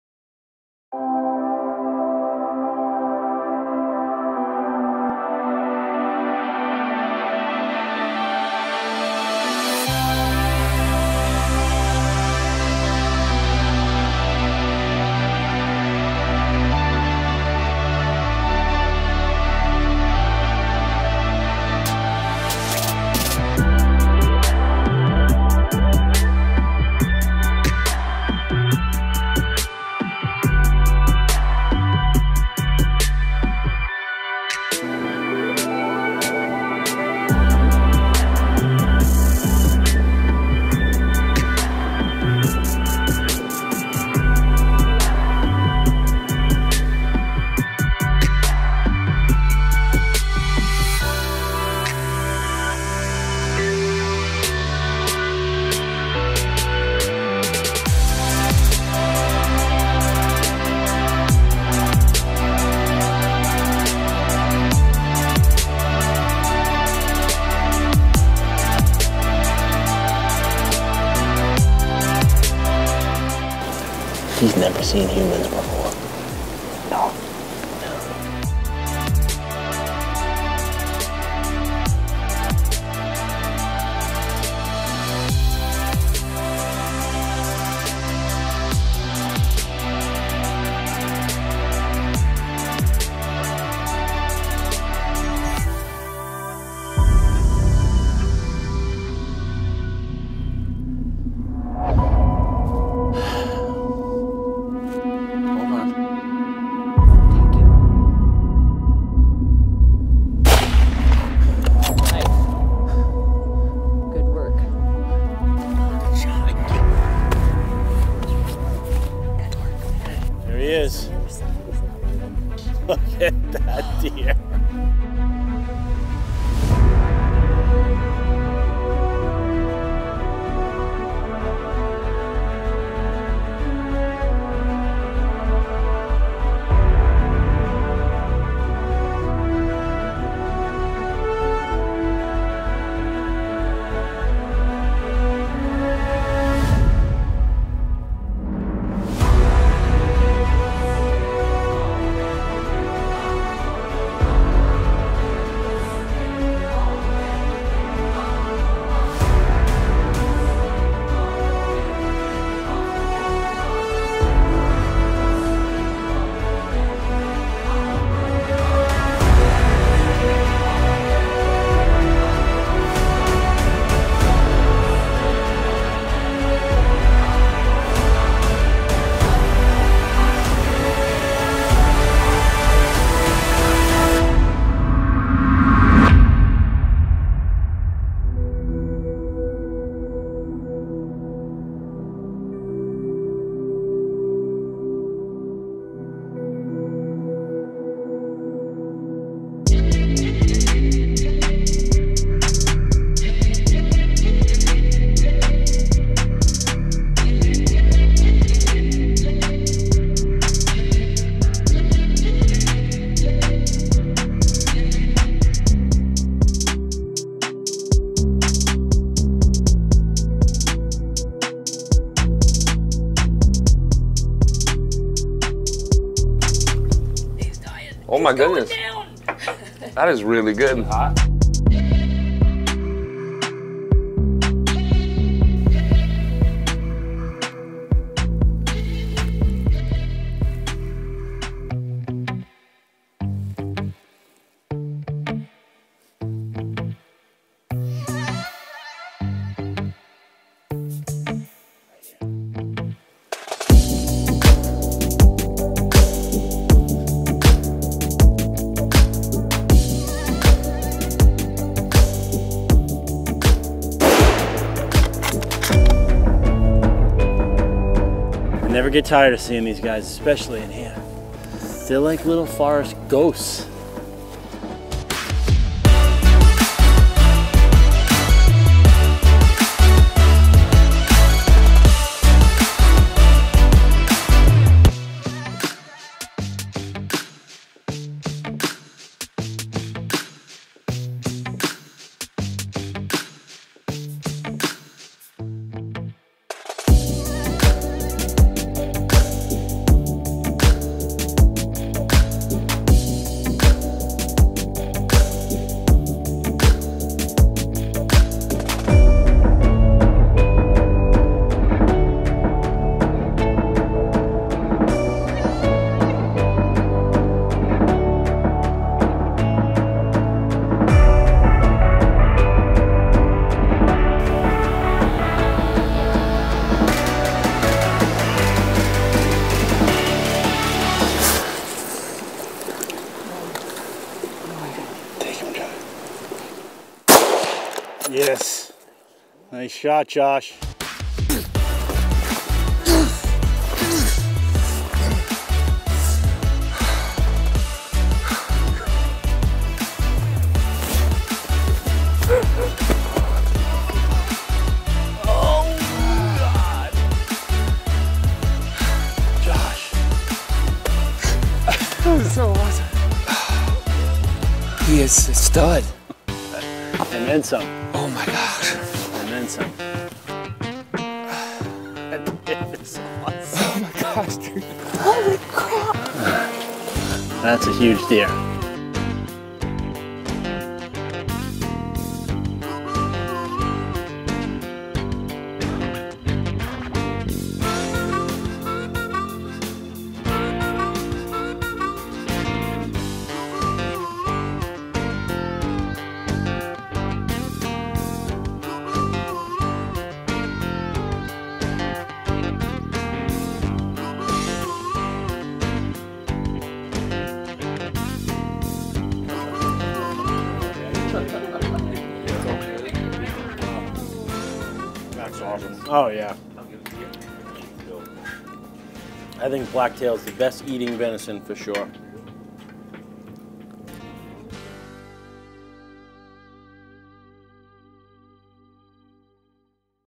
Sitka Blacktail Deer Hunting in Southeast Alaska with Mutts Decker of Glacier Guides inc.
Hunting Sitka Blacktail with the crew of Swarovski optics and HS Percision what’s outfitter Glacier Guides inc.